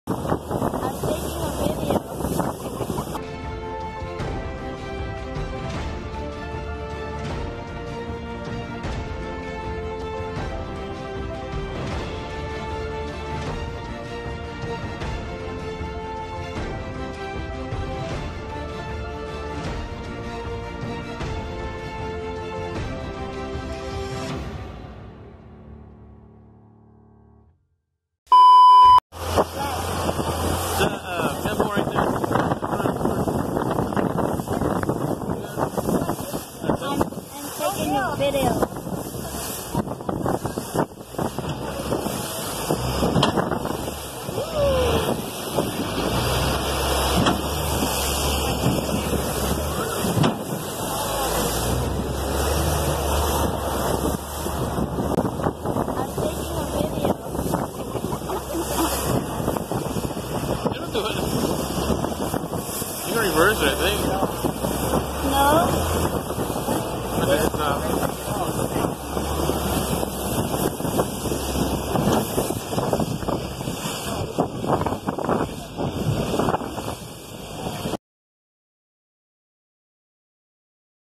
Americans First Time to Ride Turbo Long Tail Boat in Thailand | Meeting the In-Laws | Vlog #8
After a long ride pag baba na hilo si mom